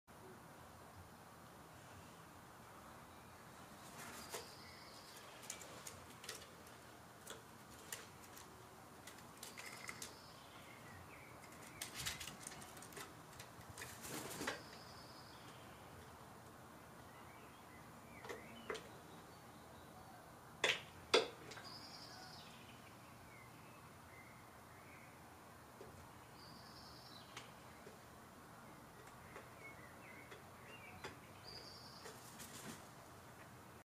European Jackdaw Gets Lots Of Nuts! (OC)
Spotted this darling on my patio, masterfully stacking a massive amount of nuts in their beak. ♡

Filmed using a LUMIX DMC-G70, stock lens.